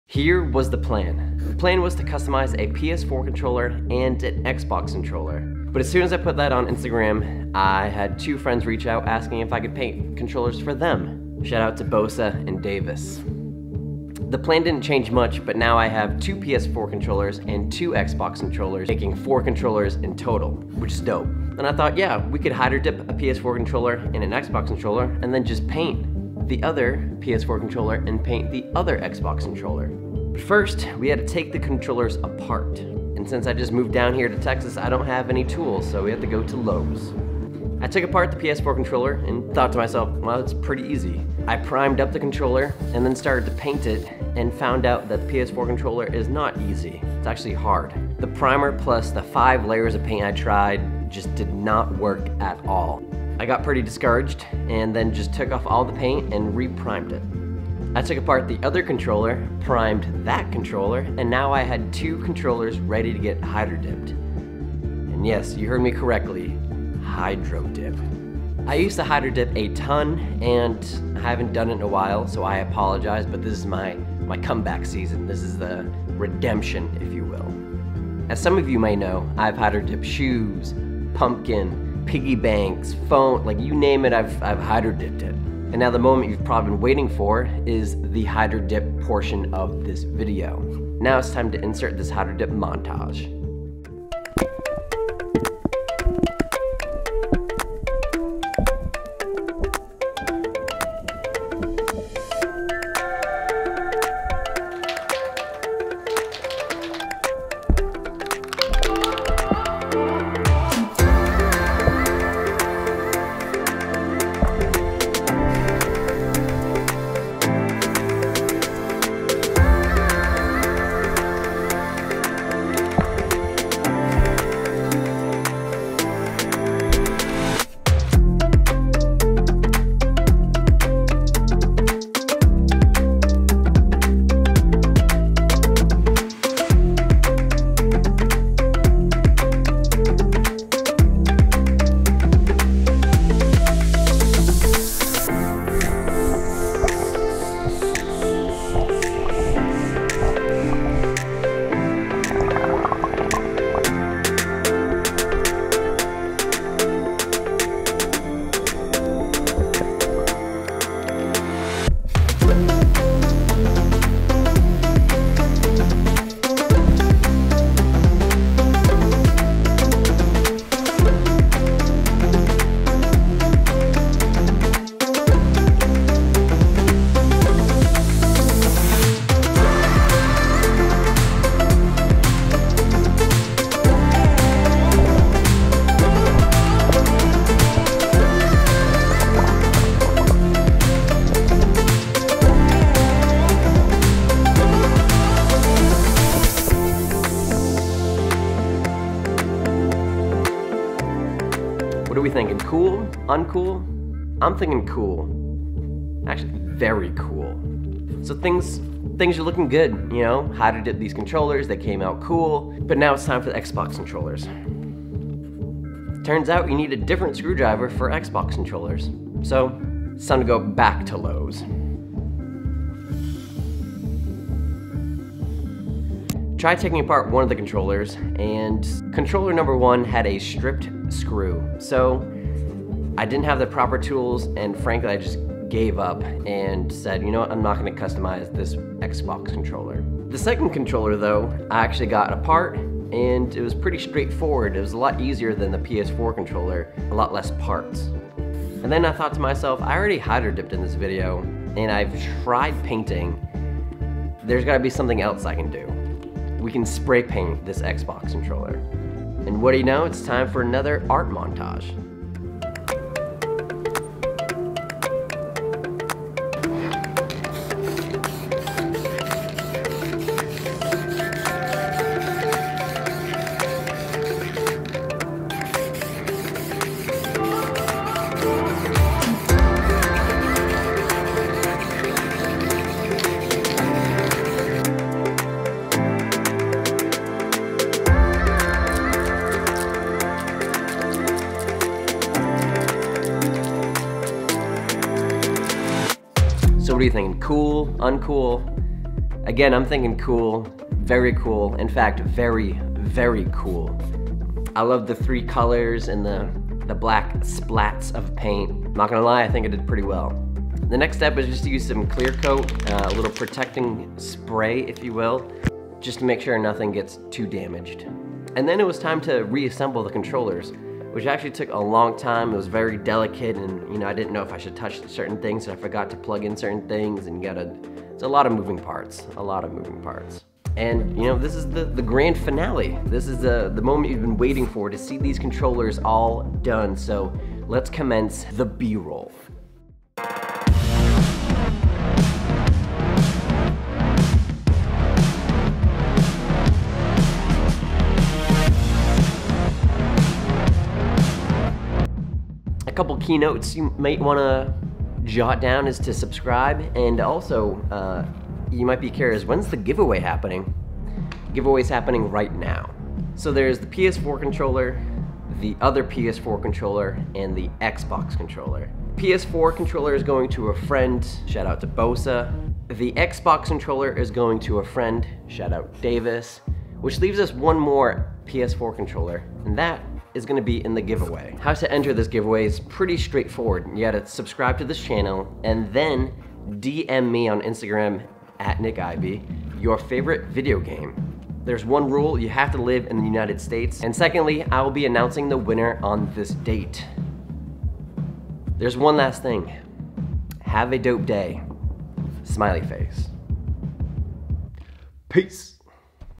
HYDRO DIPPING PS4 CONTROLLERS 😮 (GIVEAWAY) !!!!
Pretty stoked on how these came out! 😊

MAIN CAMERA
DJI OSMO POCKET
SPRAY PAINT
PAINT PEN (I use black)